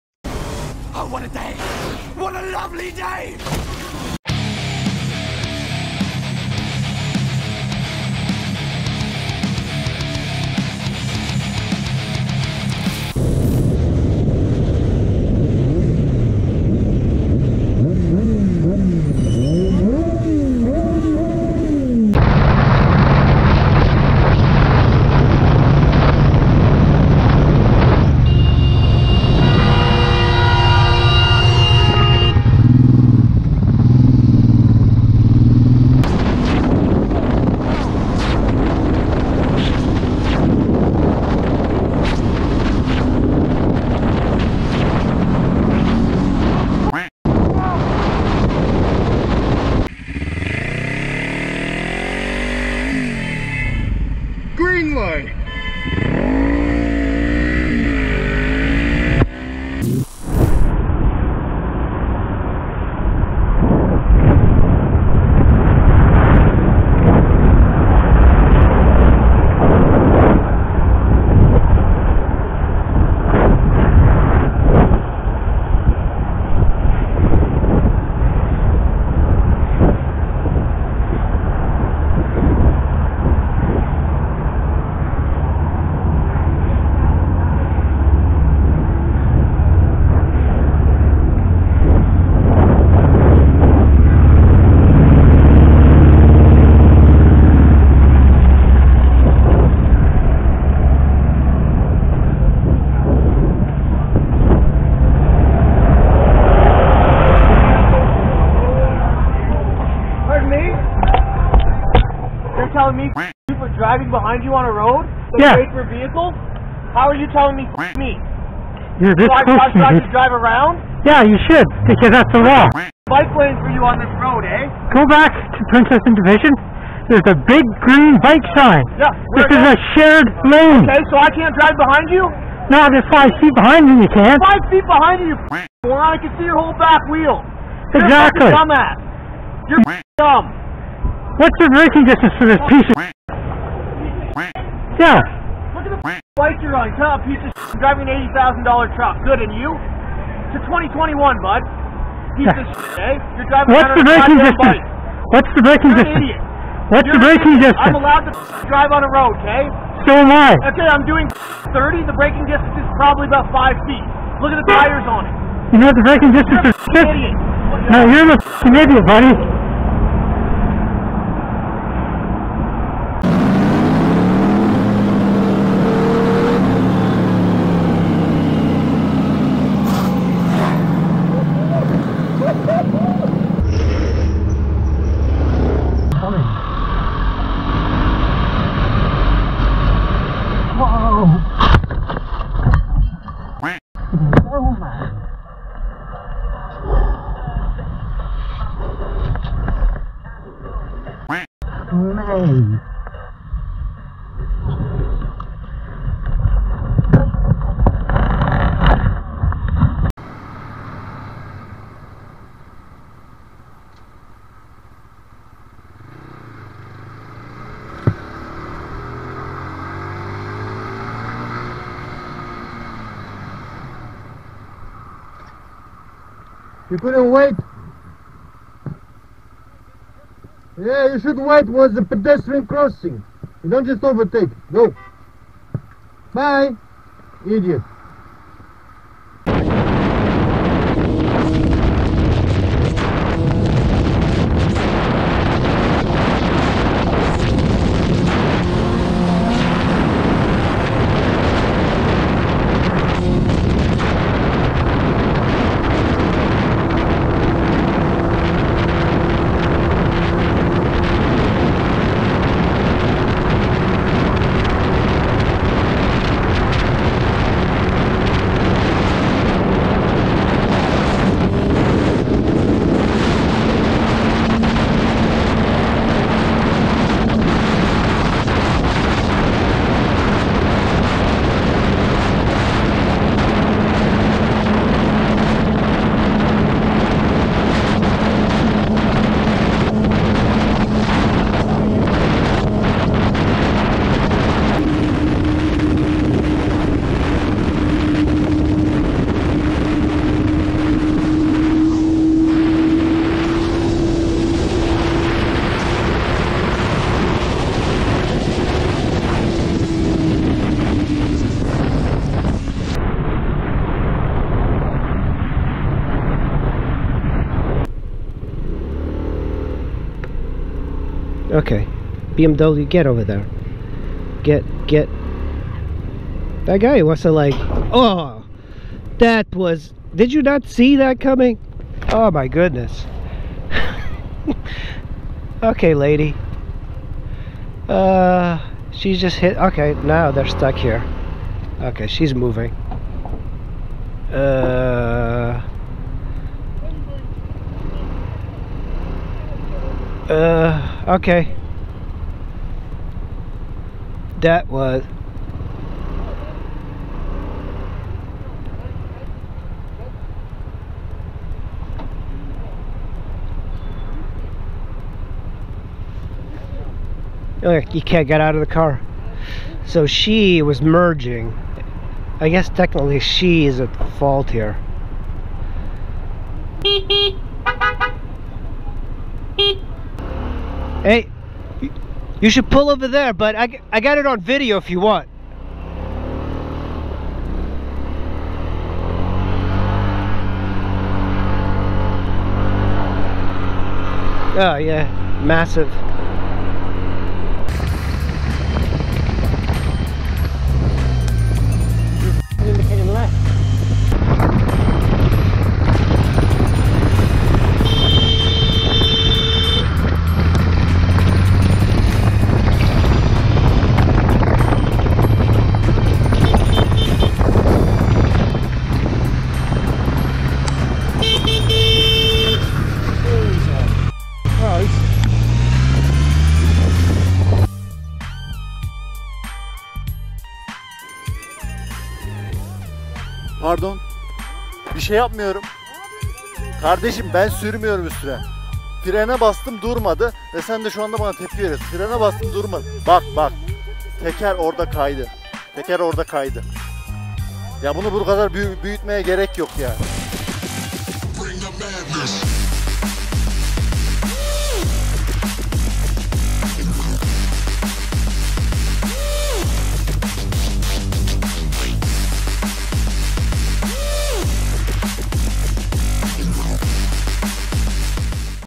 STUPID, CRAZY & ANGRY PEOPLE VS BIKERS 2021 [Ep.#1019]
★ SPECIAL THANKS 2 THESE YOUTUBER'S WHO SUPPLIED CLIPS! SHOW YOUR SUPPORT AND GIVE THEM A SUB! ➡
[00:00] ➤ INTRO
[00:13] ➤ Hector Paulino - Almost Got Hit by a Bus
[00:22] ➤ K Richardson - Close Call for Biker
[00:35] ➤ Ryan Wipfield - Driver Gets in My Lane for the Second Time
[00:49] ➤ Anthhimself - Near Miss
[01:00] ➤ Anonymous
[02:53] ➤ Ndthien - Near Crash Yamaha MT09
[03:06] ➤ Buddy the Brit - Motorcycle Accident
[03:30] ➤ Raven Rider - Overtaking on Pedestrian Crossing
[03:59] ➤ Aristhrotle - Weird Road Rage
[04:55] ➤ Butter Balls - Close Call
[05:18] ➤ Motard Squid - Who's Fault?
[06:37] ➤ Chris B - More Idiot Drivers
[07:07] ➤ Tarik Erdem - KAVGA ÇIKARAN ADAMLAR 1 (MOTOSİKLET VİDEOLARI)

Moto Madness 2021